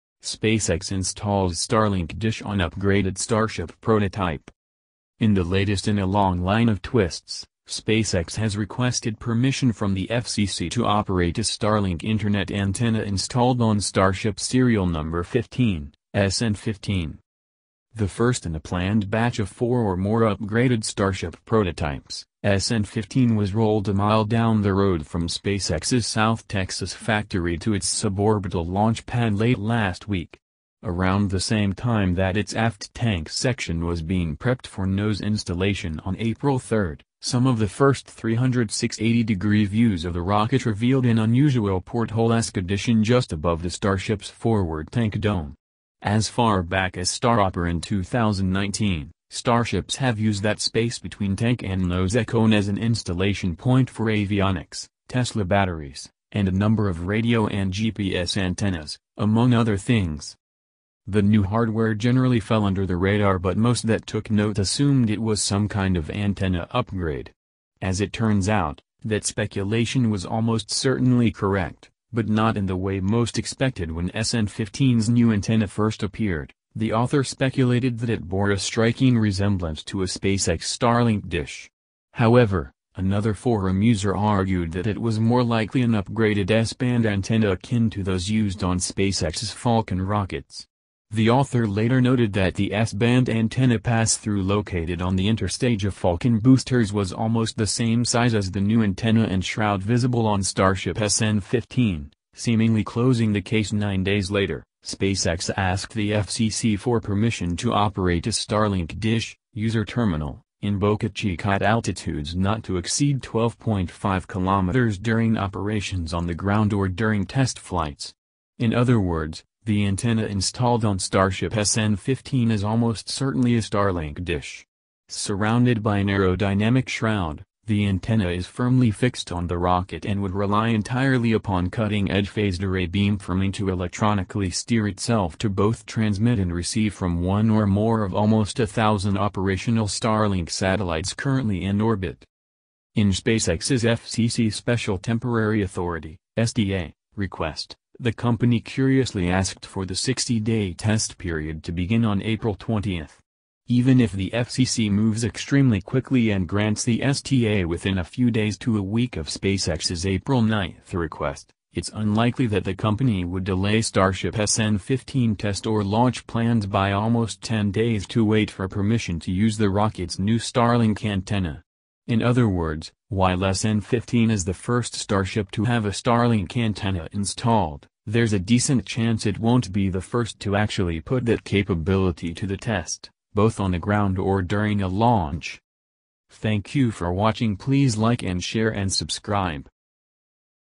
SpaceX is installing the Starlink dish on the upgraded Starship prototype
Get free after effects and premiere pro templates

In the latest long string of flips, SpaceX has requested permission from the Federal Communications Commission (FCC) to operate a Starlink Internet Antenna installed on the Starship 15 (SN15) serial number.

The first in a planned batch of four or more upgraded spacecraft models, the SN15 was rolled a mile from SpaceX’s plant in South Texas to a sub-orbital launch pad late last week. Around the same time that the rear tank section was equipped for a nose mount on April 3, some of the first 360-degree sightings of the missile revealed an unusual addition to the hatch above the Starship’s front tank dome. As far as the Starhopper in 2019, the Starships used that space between the tank and the nosecone as an anchorage for avionics, Tesla batteries, and a number of radio and GPS antennas, among other things.

New devices generally fall under the radar, but most of those who took note assumed it was some kind of antenna upgrade. As it turns out, this speculation was almost certain to be correct – but not in the way we had expected so much.

Starship SN15 is set to begin its qualifying test drive early Monday, April 12th. (NASASpaceflight – bocachikagal)
When the new SN15 antenna debuted, The author speculated It bears a striking resemblance to a SpaceX Starlink dish. However, another forum user argued that it was likely an upgraded S-band antenna similar to the one used on SpaceX’s Falcon rockets. The author Later observed The passage of the S-band antenna on the interchange of the Falcon boosters was roughly the same size as the new antenna and the visible cover on the Starship SN15, apparently closing the case.

The Falcon 9 B1060 Telemetry Antenna and the previously enigmatic Starship SN15 Antenna are roughly the same size. Both missiles are widespread here. (Richard Angel / NSF-Bokachikagal)
Nine days later, SpaceX asked the FCC for permission to operate a Starlink dish (user station) in Boca Chica “at altitudes not exceeding 12.5 km” during operations “on the ground or during test flights.” In other words, the antenna installed on the Starship SN15 is almost certain to be the Starlink dish. Surrounded by an aerodynamic hood, the antenna is securely attached to the missile and will rely entirely on a sophisticated advanced beam formation to electronically orient itself for both transmission and reception from one or more of the nearly a thousand satellites currently in operation at Starlink in orbit.

In the Federal Communications Commission’s (STA) special request from SpaceX, the company kindly requested that the 60-day probation period begin on April 20. Even if the FCC moves very quickly and awards STA within a few days to a week of SpaceX’s April 9 request, the company is unlikely to delay testing the Starship SN15 or launch plans for nearly ten days pending permission to use the new missile. Starlink Antenna. In other words, while the SN15 is the first spacecraft with a Starlink antenna installed, there is a good chance that it will not be the first to actually test this capability – either on Earth or during launch.

Although unlikely, the fact that SpaceX’s Starship SN15 Starlink antenna fitting is roughly the same size as the reliable but much more basic S-band setup than Falcon’s, which also raises the question of whether or not the vehicle’s test flights succeed Space may eventually lead to the addition of Starlink dishes on future Falcon boosters. Regardless, Starship SN15 is on its way to start a busy week of qualification exams in South Texas. If the missile suffers any major delays, as is typical of the Starship’s prototypes, there is a good chance that the SN15 will begin testing its Starlink dish in the middle of next week.